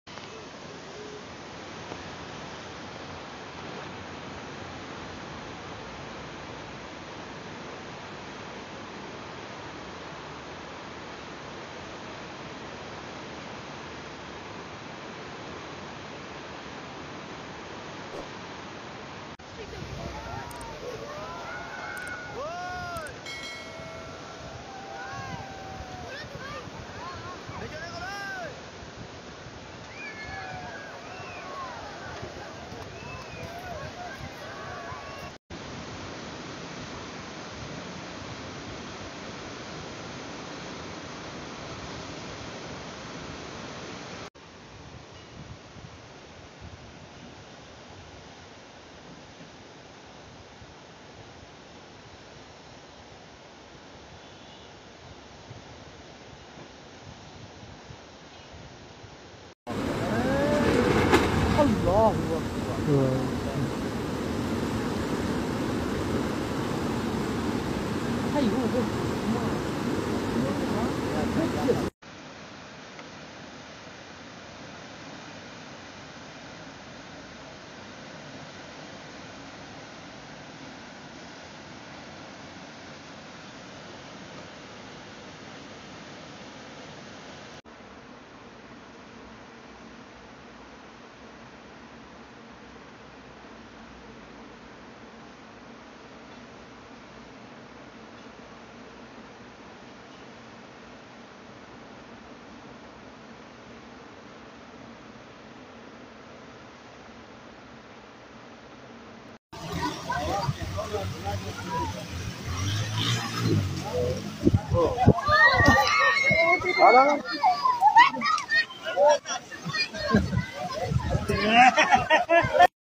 Suddenly dangrous flood apears veachicles will get stuck.| no copyright video channel. New video.
Crossing vehicles will get stuck in water.
Due to suddenly foold appears people will get
stuck. dangerous flood in rivers. everythings will destroy in near  rivers.
Dangrous
flood
no copyright